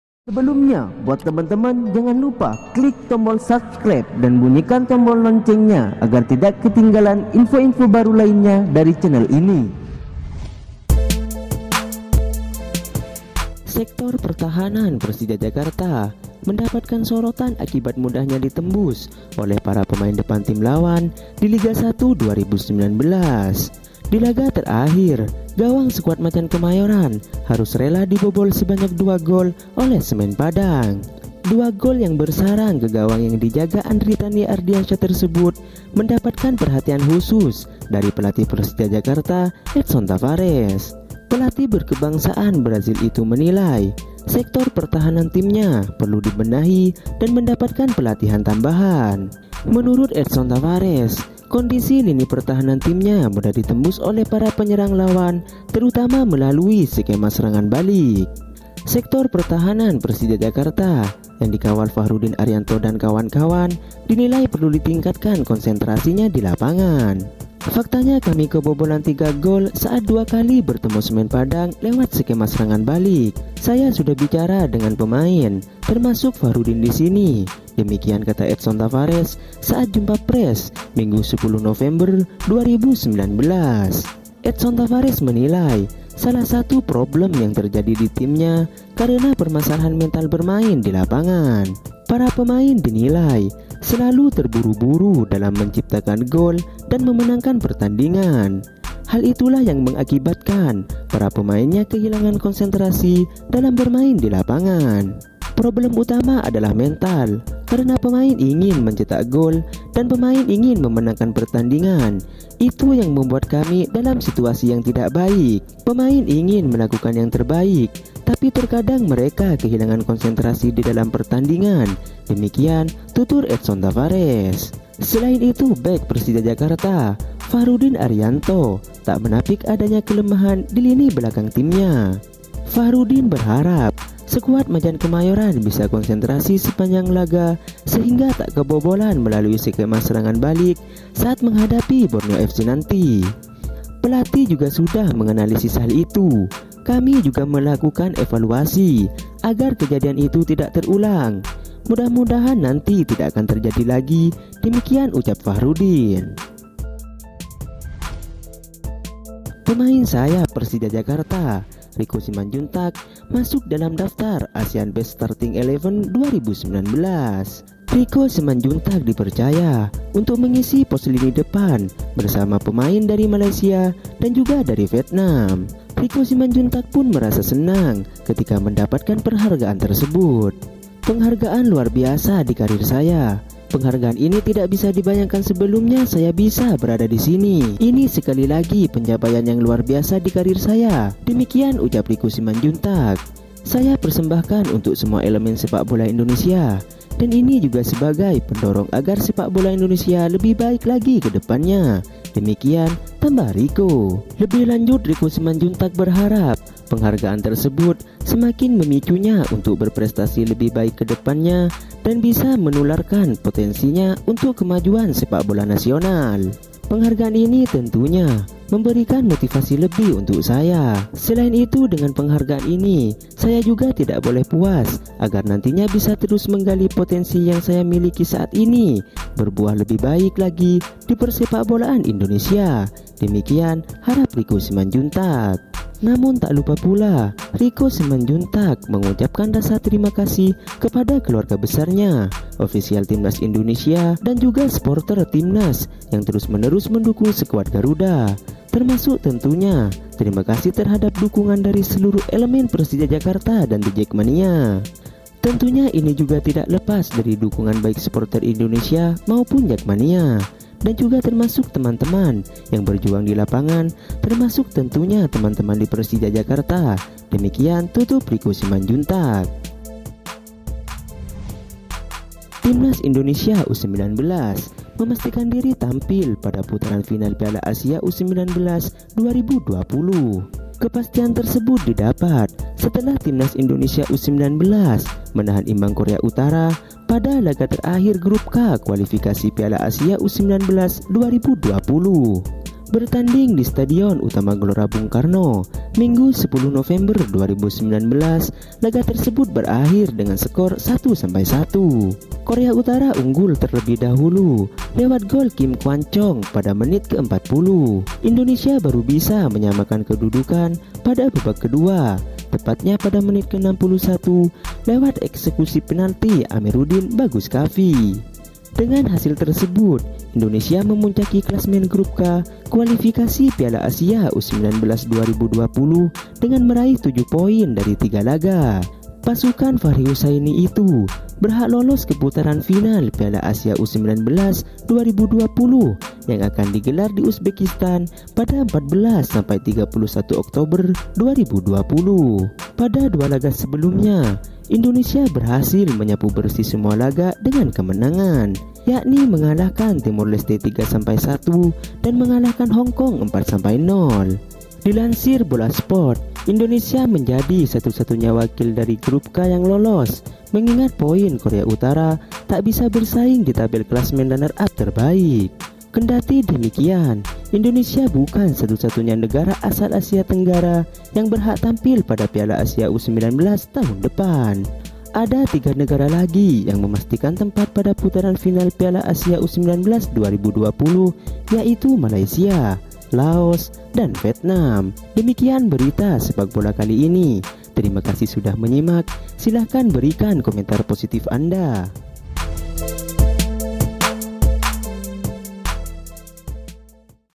Edson Tavares Menyoroti Lini Pertahanan Persija,Indonesia U19 Lolos Ke Final Piala Asia 2020!
" CAUTION!! "
                ¤ BIASAKAN NONTON SAMPAI HABIS ¤
           ¤ AGAR TIDAK SALAH DALAM MEMAHAMI ¤

3 Berita Bola 11 November 2019!
===》Sering Kebobolan! Edson Tavarea Menyoroti Lini Pertahanan Persija!
===》Riko Simanjuntak Masuk Daftar Asean Best Starting XI 2019

Foto : INTERNET.

NB : FOTO-FOTO DI ATAS HANYA BAHAN YANG KAMI PAKAI UNTUK MENJELASKAN INTI DARI ISI BERITA YANG KAMI BAWAKAN.

                                         " CAUTION!! "